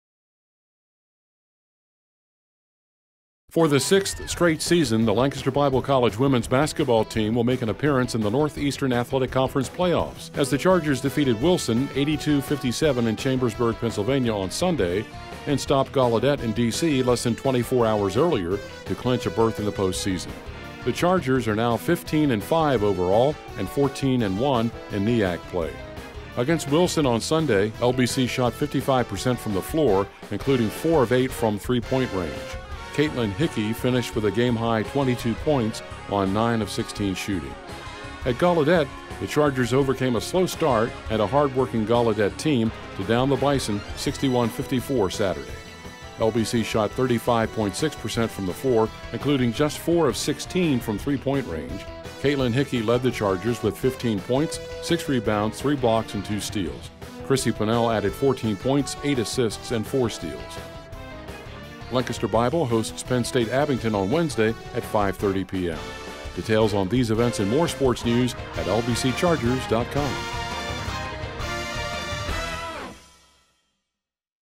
WBB: Lancaster Bible Weekend vs. Wilson & Gallaudet - Highlights
The Lancaster Bible College Women's Basketball team swept the weekend competition vs. Wilson on Sunday 05 February 2017 and Gallaudet on Saturday 04 February 2017 to earn a playoff spot in the NEAC.